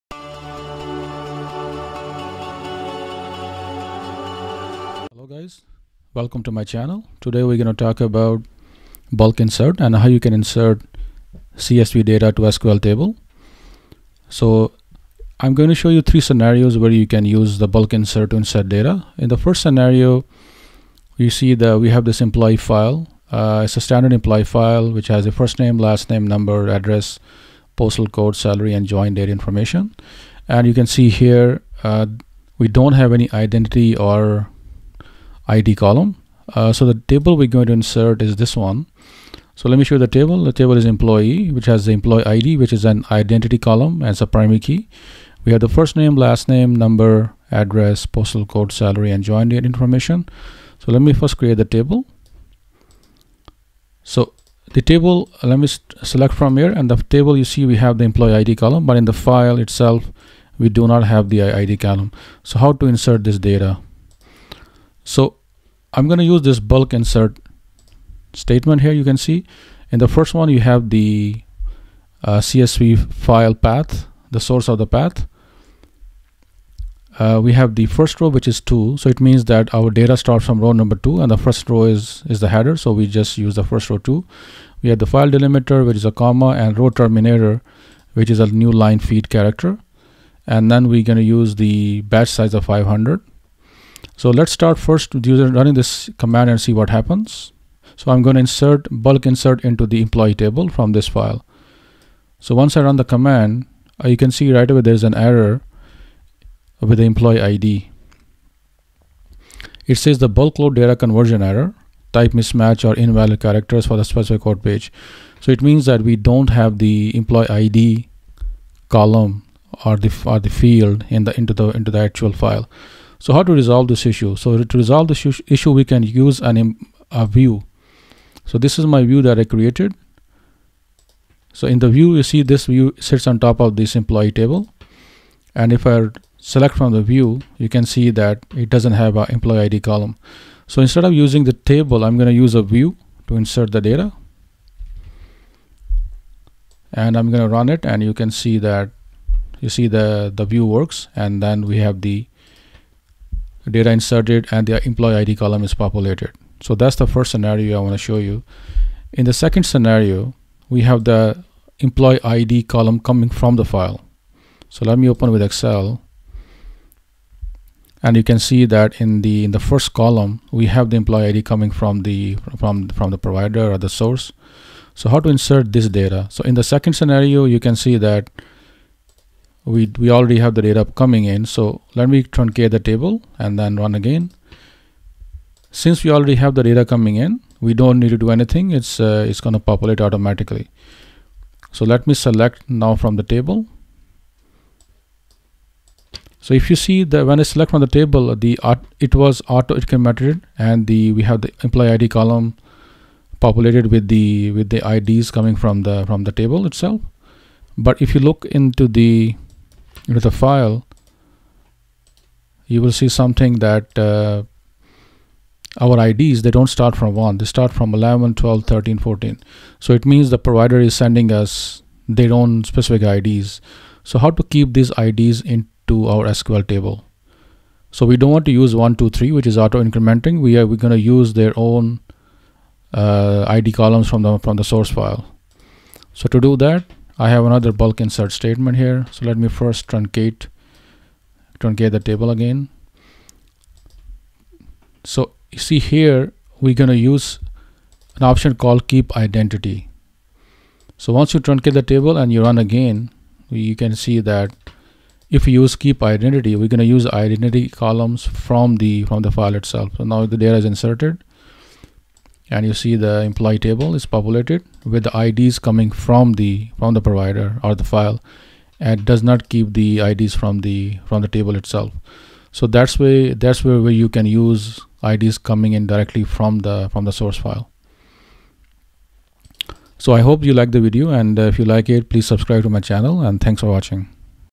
Bulk Insert CSV to Sql Table - Keeping Identity Column with Three Scenarios
CODEWITHSHAZ. Bulk Insert CSV file to SQL table. This video shows you how to insert csv data and keep the SQL Identity column. It shows three scenarios. One file which does not have the Id column and the other ones which have the Id columns.